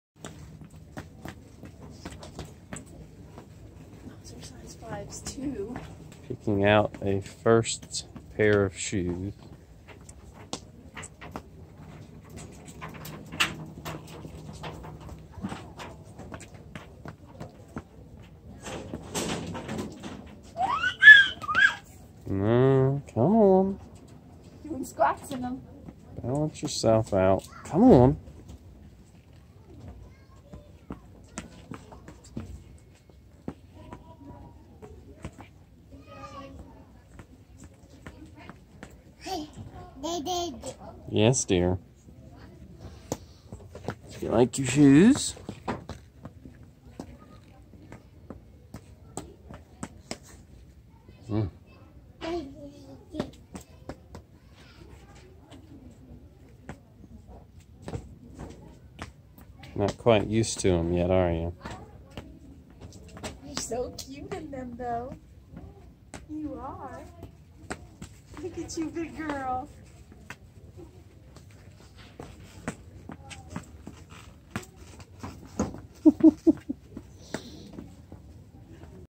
Emma Shoe Shopping - 20190105
First Rubber Sole Shoes.  She took off several times.  Didn't want to go back in the cart.